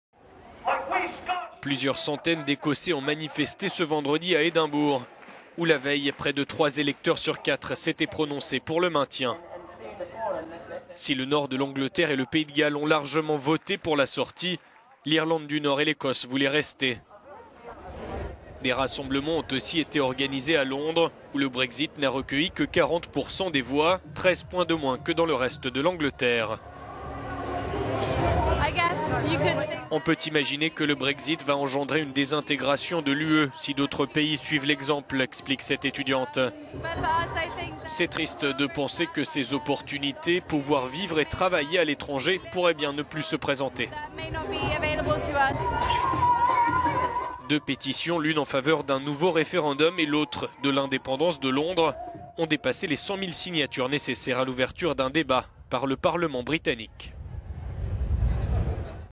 Rassemblements anti-Brexit à Londres et Édimbourg
Plusieurs centaines d’Écossais ont manifesté ce vendredi à Édimbourg, où la veille près de trois électeurs sur quatre s'étaient prononcé pour le maintien du Royaume-Uni dans l'UE. Si le nord de l'Angleterre et le Pays de Galles ont largement voté pour la sortie, l'Irlande du Nord et l’Écosse voulaient rester. En Écosse, le "remain" a recueilli 62%.

Des rassemblements ont aussi été organisés à Londres, où le Brexit n'a recueilli 40% des voix, soit 13 points de moins que dans le reste de l'Anglet…